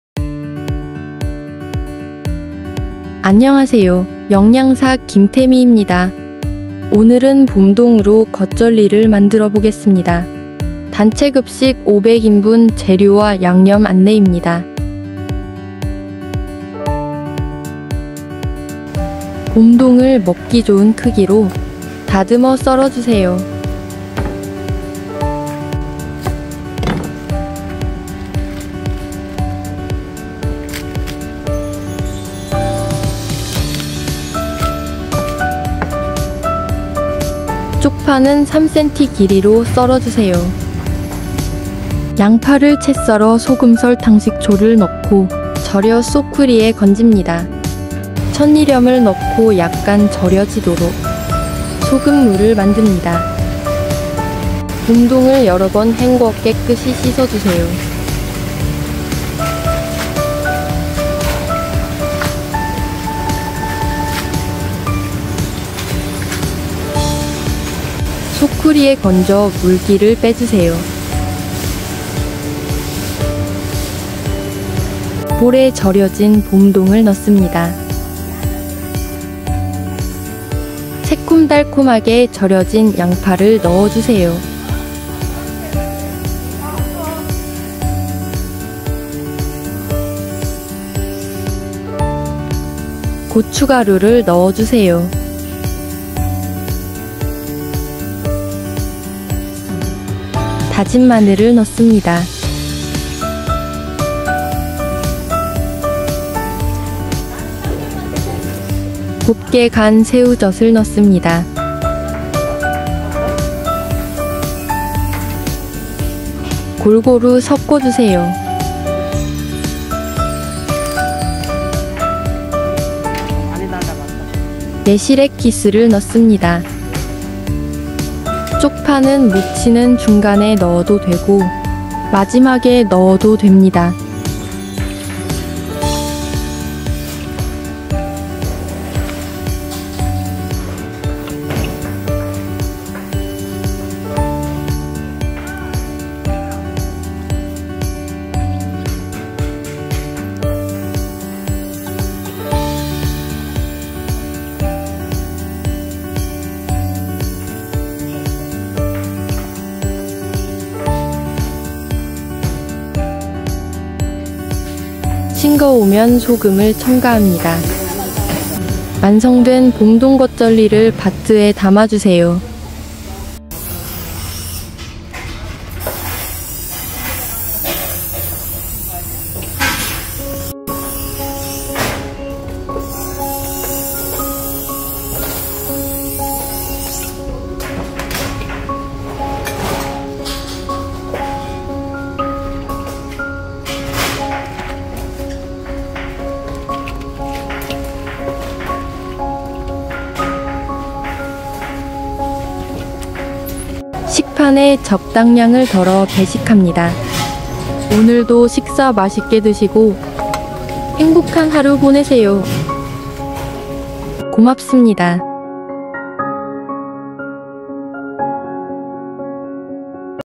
11월 부터 제철인 봄동겉절이 드셔보세요#단체급식
봄동 영양성분 (100g)
칼로리 21 kcal
탄수화물  4.59g
단백질  1.5g
지방 0.08g